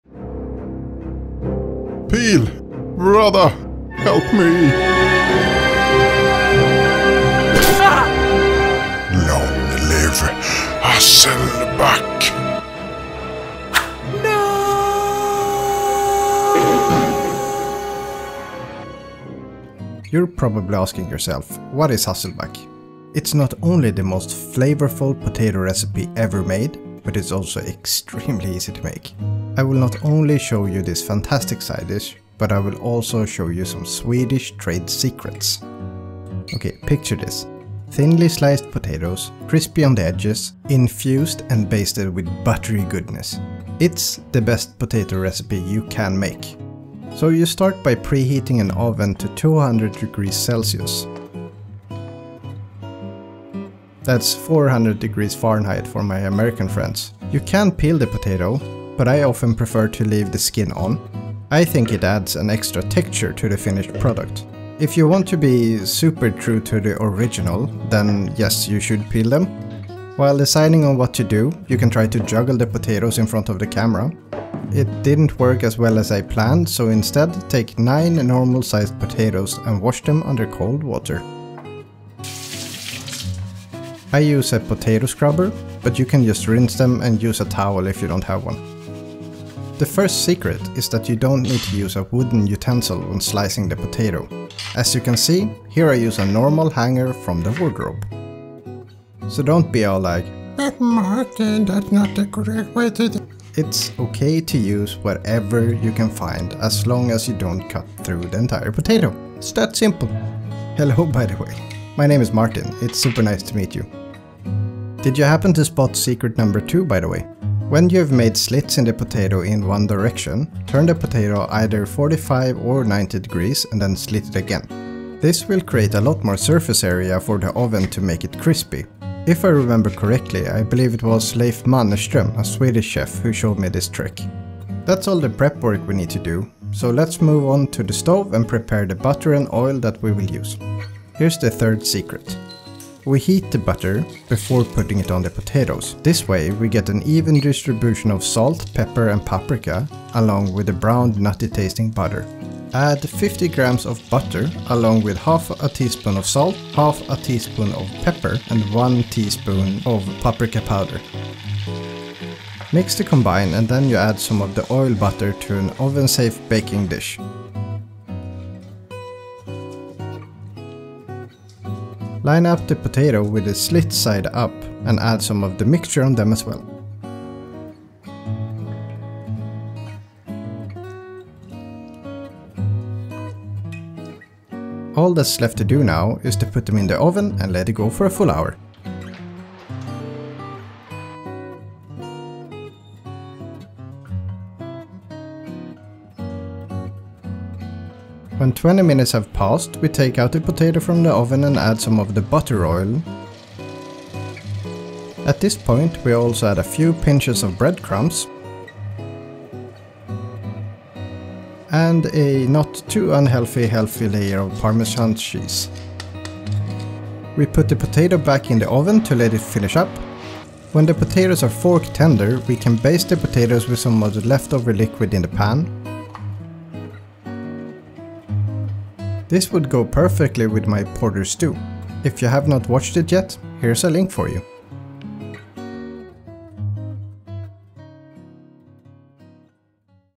How to Make Crispy Hasselback Potatoes | The Best Potato Side Dish
Discover the magic of Hasselback Potatoes. A Swedish gem that promises crispy perfection with every bite. In this recipe I'll guide you through the process while sharing insider tips and tricks to this classic potato dish. Picture thinly sliced potatoes, crisped to perfection, and infused with a buttery greatness. It's not just a side dish; it's a masterpiece waiting to be devoured.